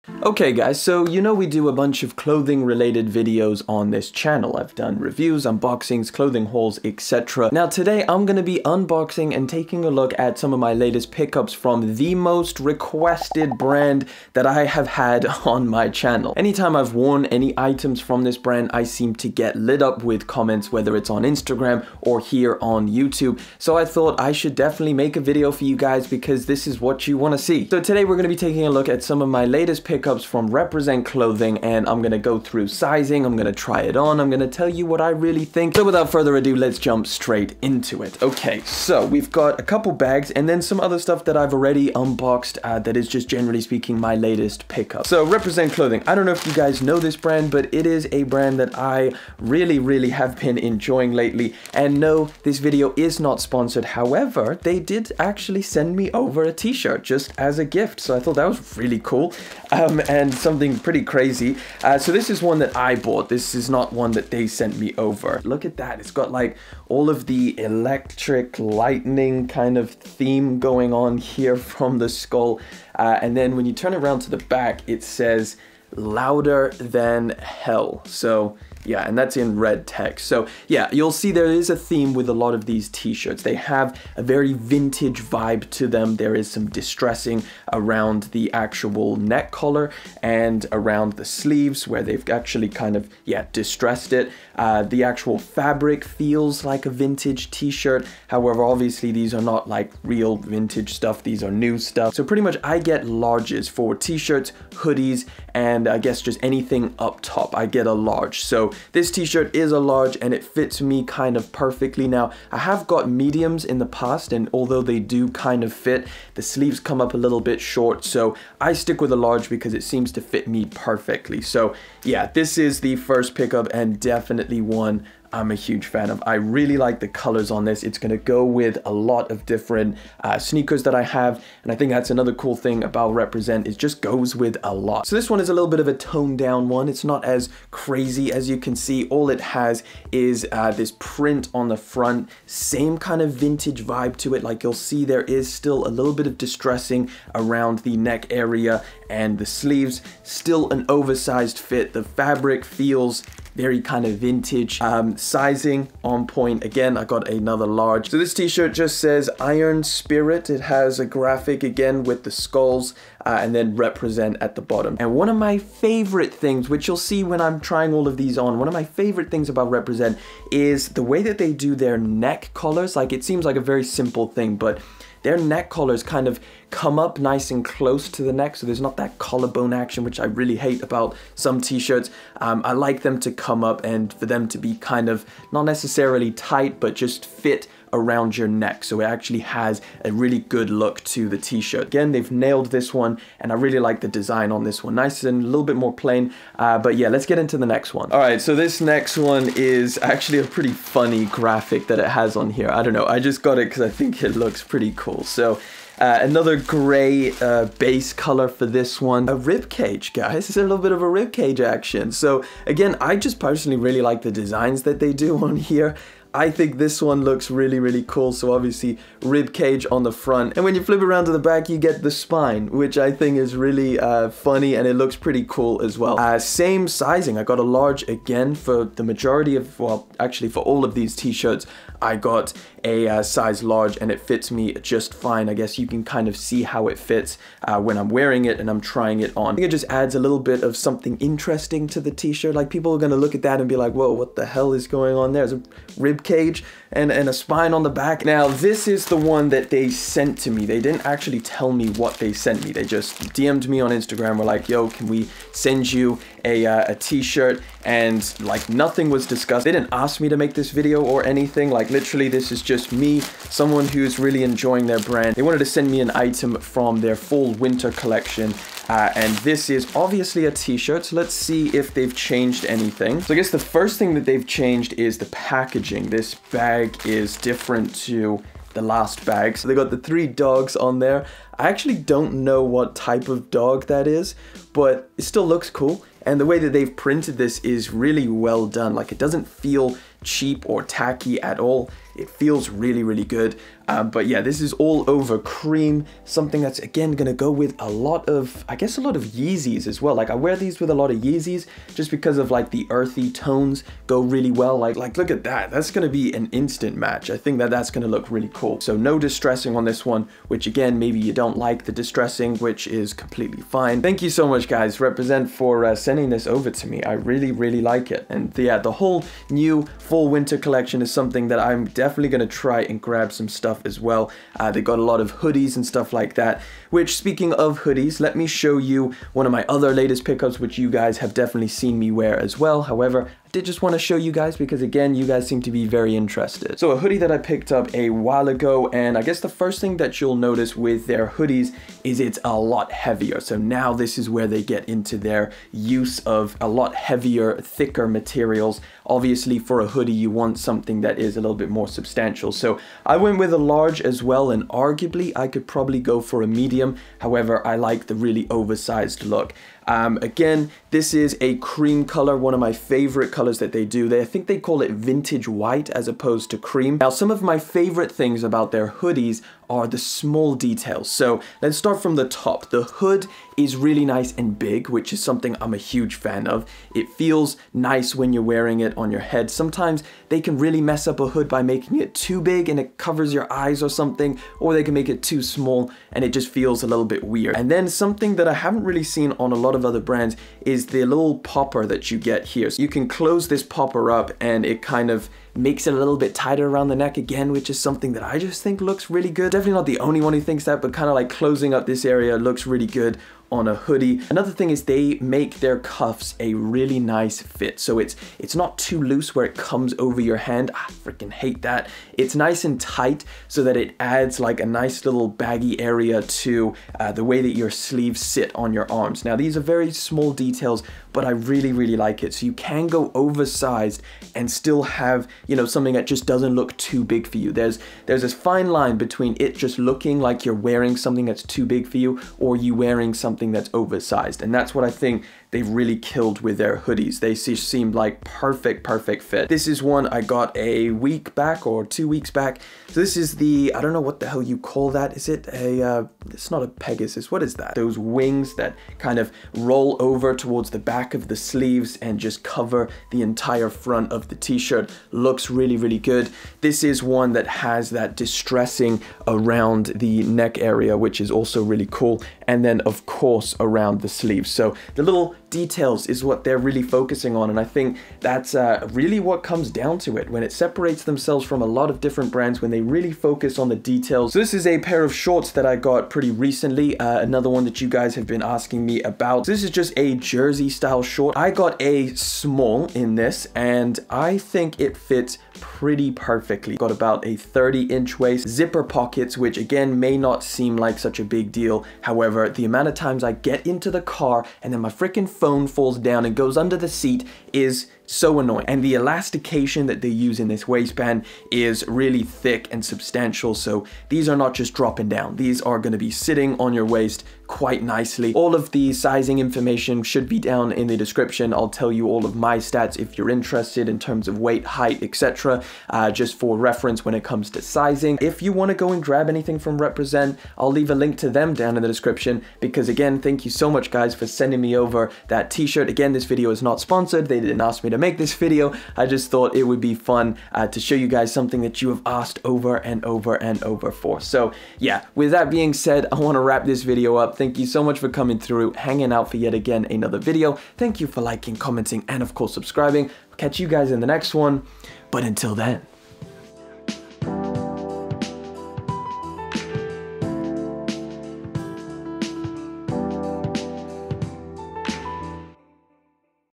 The BEST Graphic T-Shirts! You Keep Asking For These!
. Today I’m unboxing my latest pickups from Represent Clothing. They have some really cool graphic tees  that i try on and go over sizing for the items i have.

Let me know what you think in the description down below!

Weight 80kg